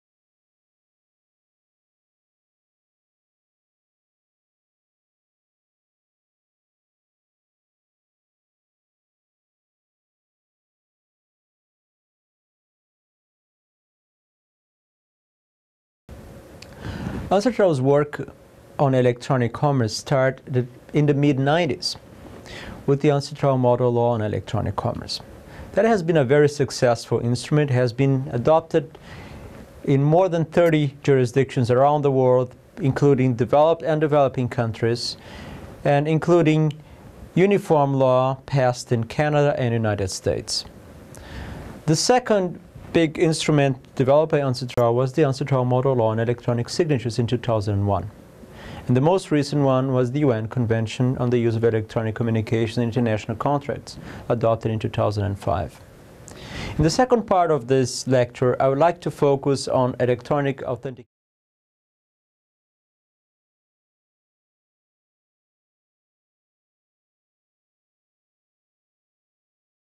José Angelo Estrella-Faria on Electronic Commerce in International Trade (Part II)
Mr. José Angelo Estrella-Faria, Senior Legal Officer, UNCITRAL Secretariat, International Trade Law Division, Office of Legal Affairs, United Nations